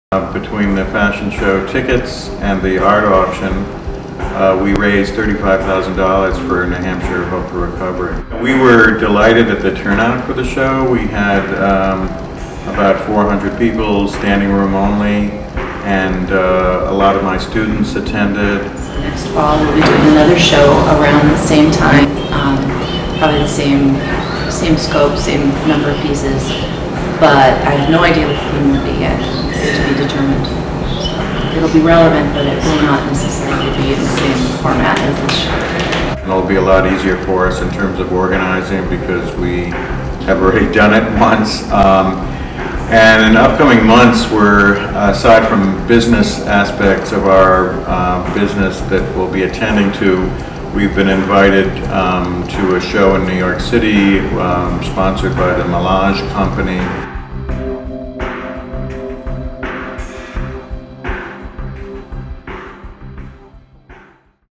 SNHU Professor Helps Create Fashion Event to Help Those Struggling With Addiction
In October 2017,  Southern New Hampshire University Professor Harry Umen and partner Laura McCarthy joined together to create "A Walk Out of the Woods," a fashion event to benefit HOPE for New Hampshire Recovery. The effort, which also featured model and SNHU alum Hailey Bonczar, raised over $35,000 to help those struggling with addiction.